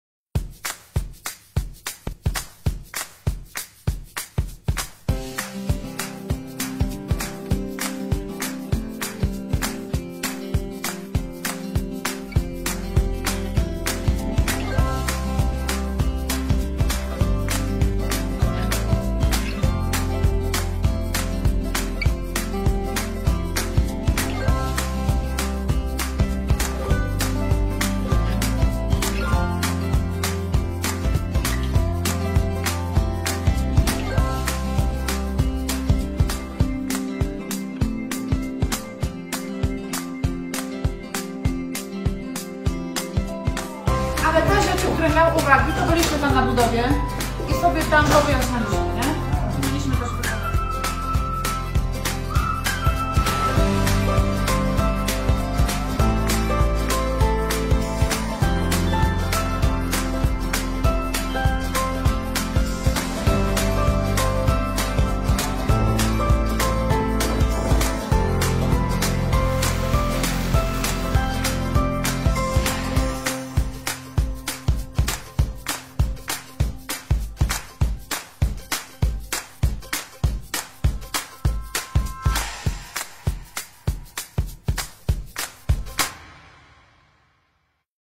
Droga w Jażyńcu
Droga w Jażyńcu z małym poślizgiem, ale została skończona jeszcze w grudniu 2018 😁.Koszt 360 tys .